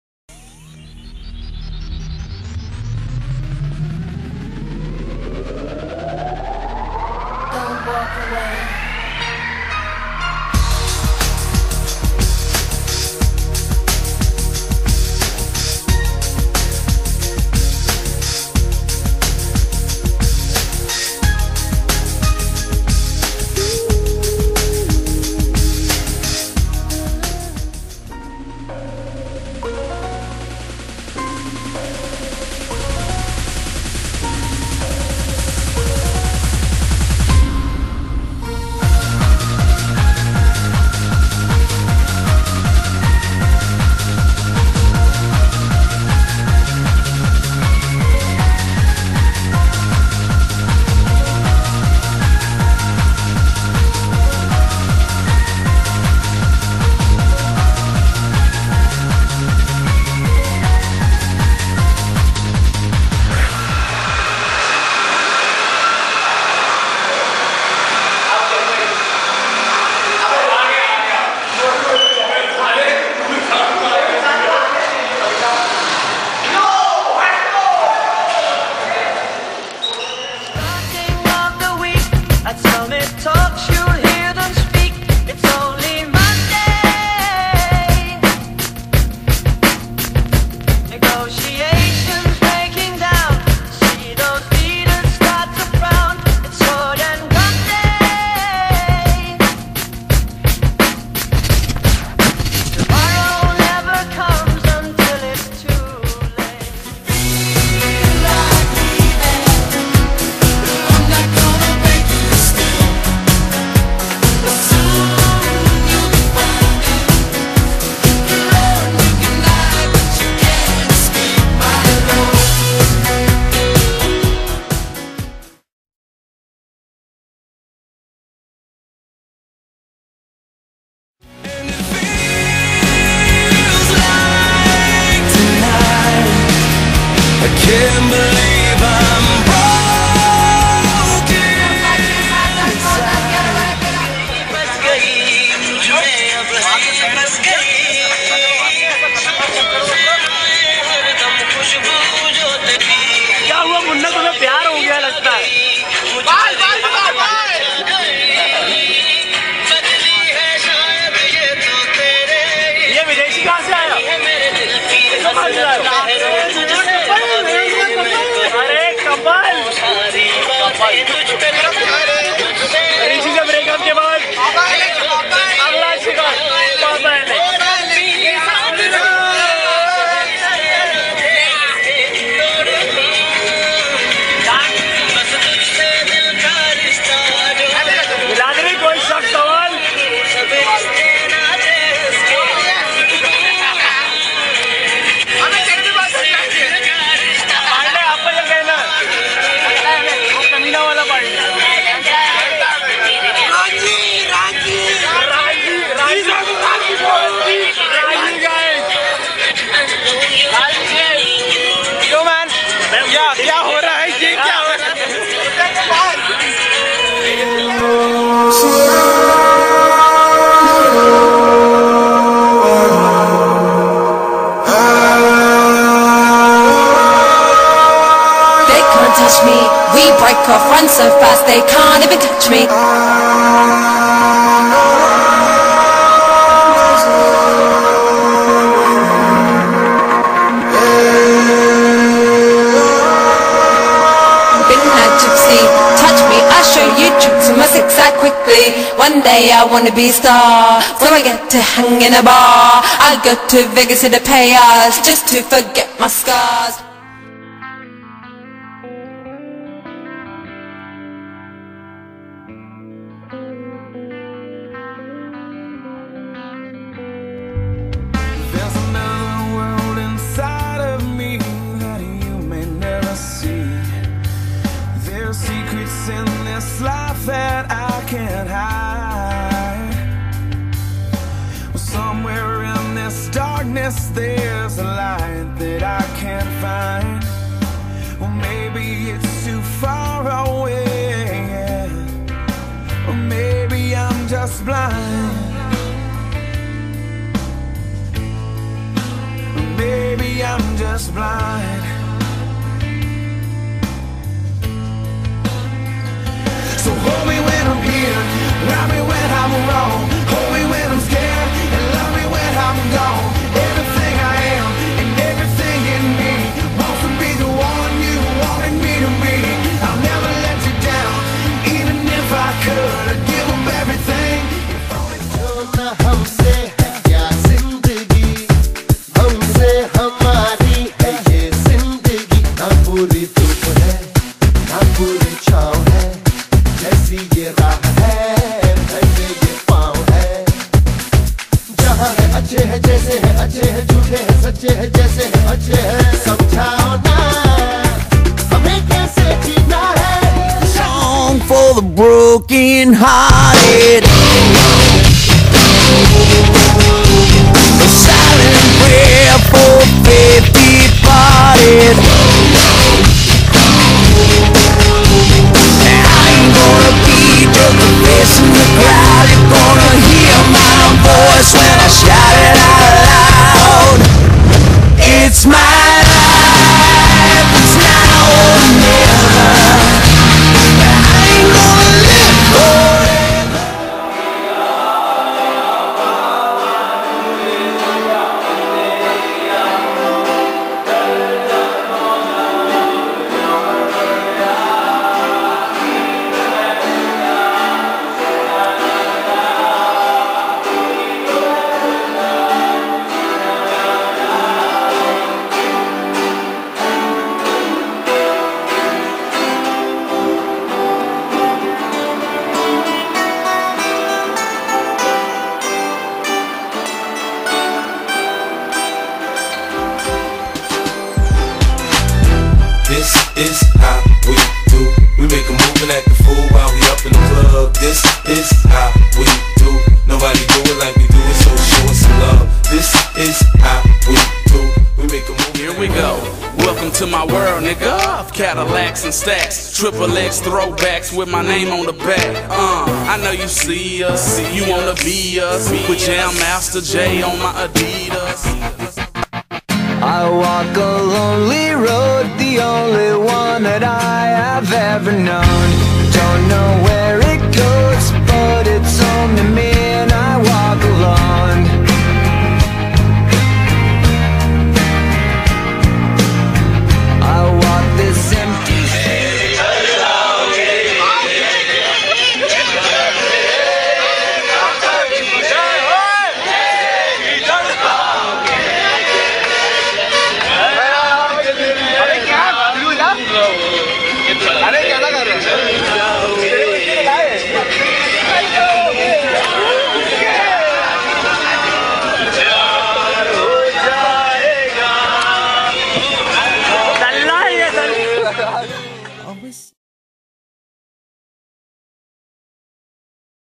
Farewell Video Part 1.wmv
HITKARINI COLLEGE OF ENGINEERING & TECHNOLOGY
Electronics & Communication Department - 2010 Batch
Farewell Party Video - Part 1 of 2
{Since YouTube is time limited for 10 minutes single extent}

A complete wrap-up of four years...